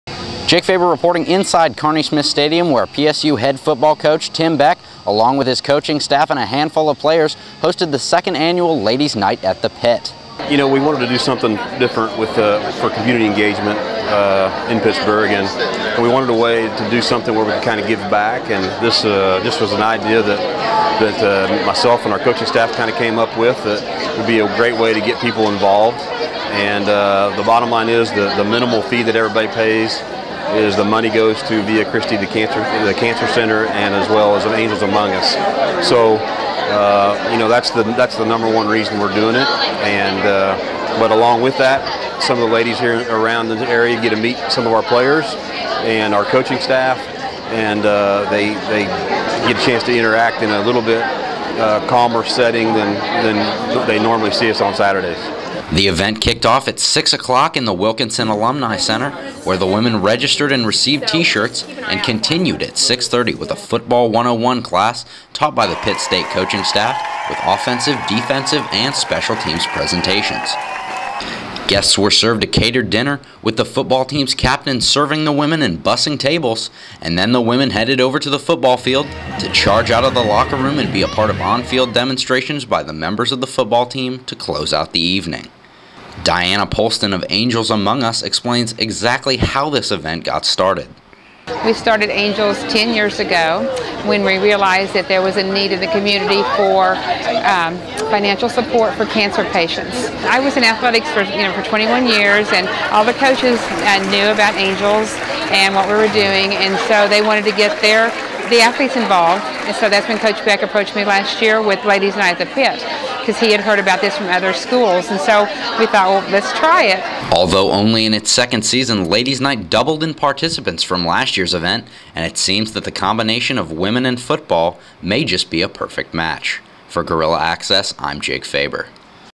Ladies Night at the Pitt
Head coach Tim Beck and the Pittsburg State University football team host the 2nd annual Ladies Night at the Pitt. Proceeds were given to the Angels Among Us Foundation and Via Christie Hospital in Pittsburg, Kansas.